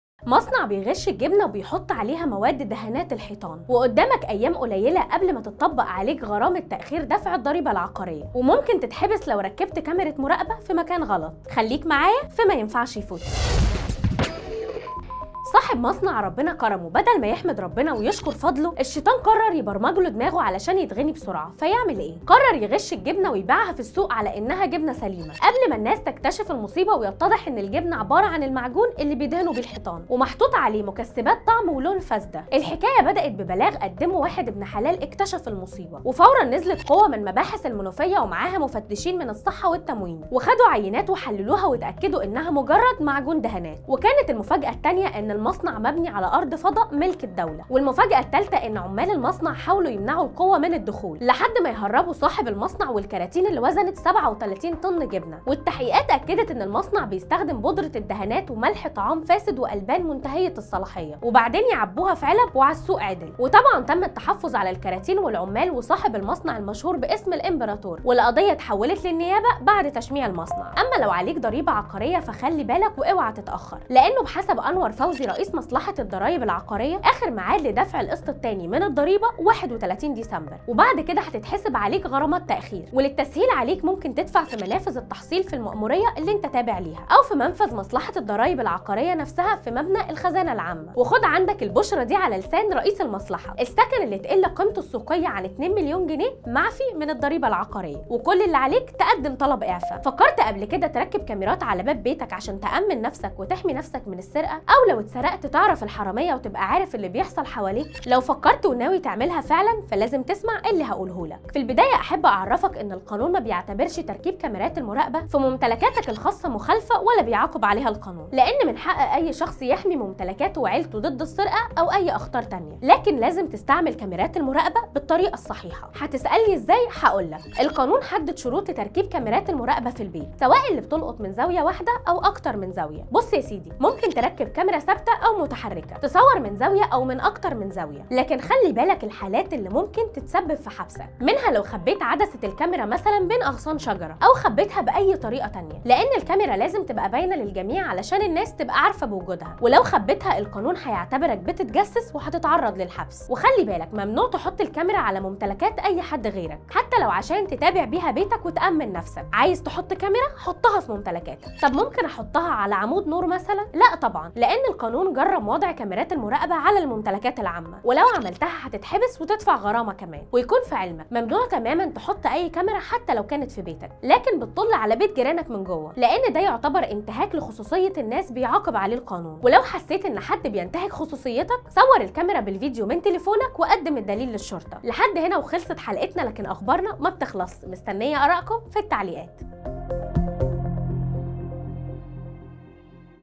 مينفعش يفوتك | سقوط امبراطور "الجبنة المغشوشة"
مينفعش_يفوتك | صاحب مصنع يخلط الجبنة بـ"دهانات الحوائط".. وكاميرا مراقبة بيتك "ممكن تحبسك"